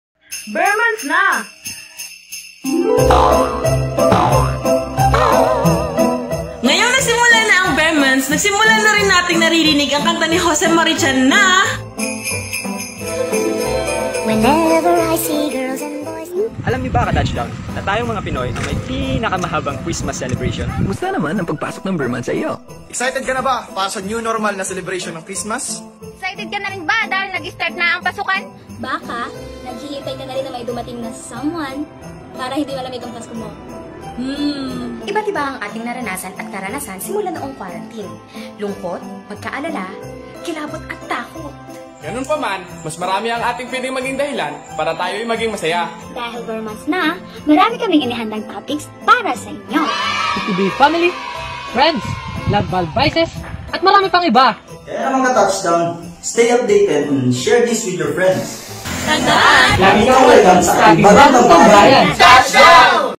TouchDown: 'BER' Months na!!
Sa kabila ng pinagdadaanan ng buong mundo ngayon, masasabi mo bang handa ka na sa darating na pasko?
Ngayong pumasok na ang taon na ito sa season na pinakahihintay ng karamihan, marami kaming inihandang topics na tiyak makakarelate ka!